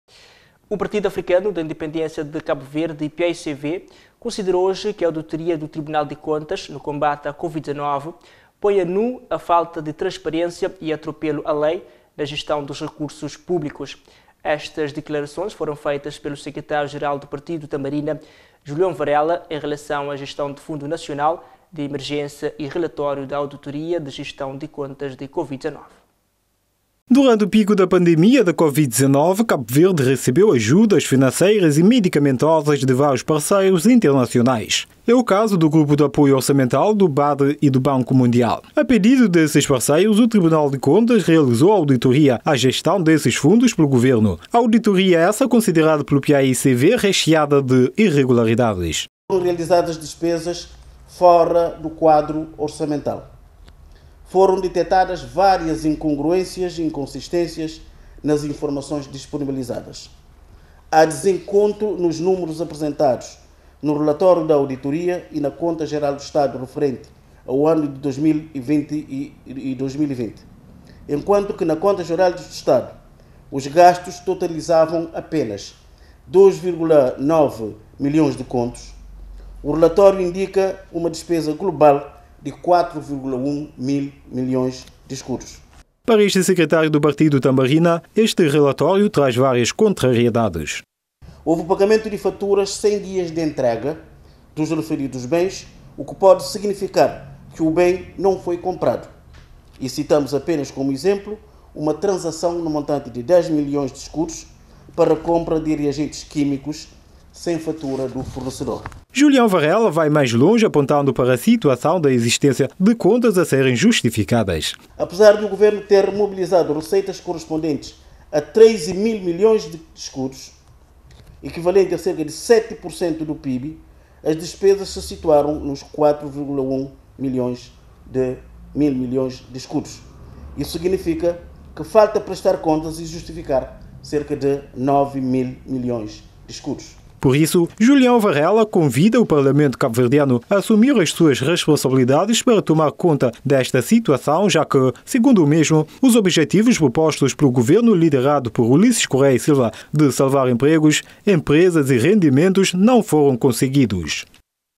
PAICV ACUSA TRIBUNAL DE CONTAS DE INTRANSPARÊNCIA
O Partido Africano da Independência de Cabo Verde, PAICV, considerou  que a auditoria do Tribunal de Contas no combate à covid-19 põe a nu, a falta de transparência e atropelo à lei na gestão dos recursos públicos. Estas declarações foram feitas pelo Secretário Geral do partido Tambarina, Julião Varela em relação à gestão do fundo nacional de emergência e o relatório da autoria de gestão às contas da Covid-19.